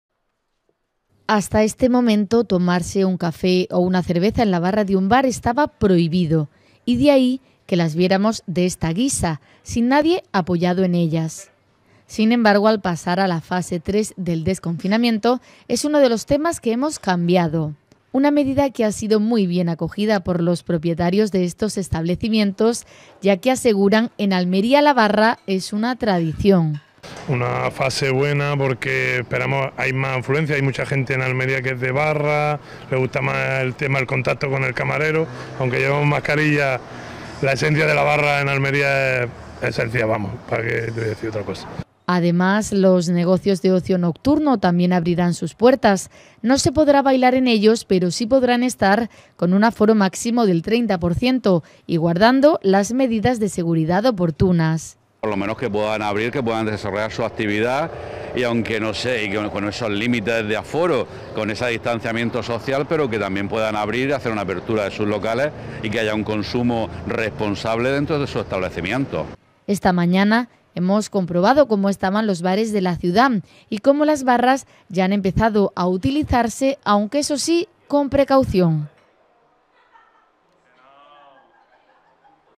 Los bares de la capital ven "un respiro" al entrar en la Fase III
La entrada a esta nueva fase conlleva una serie de mejoras para el sector hostelero, una de ellas es que a partir de ahora se pueden utilizar las barras de los bares en toda nuestra comunidad autónoma.